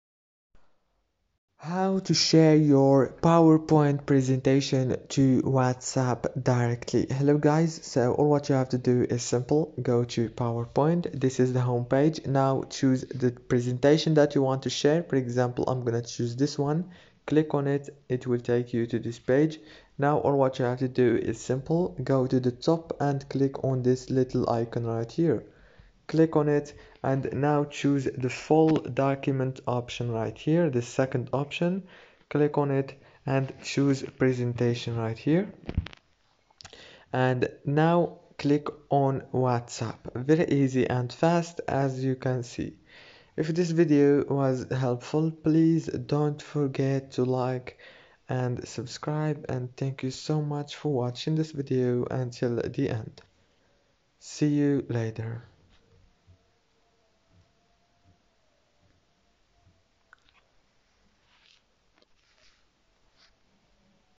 How To Share Presentation Document on Microsoft Powerpoint PPT To Whatsapp Mobile App
My idols on YouTube:
Mike Vestil
Ali Abdaal
biaheza
Kevin David
Incognito Money
Sara Finance
Everyday Money
Channel Makers
Vidiq
SuperHumans Life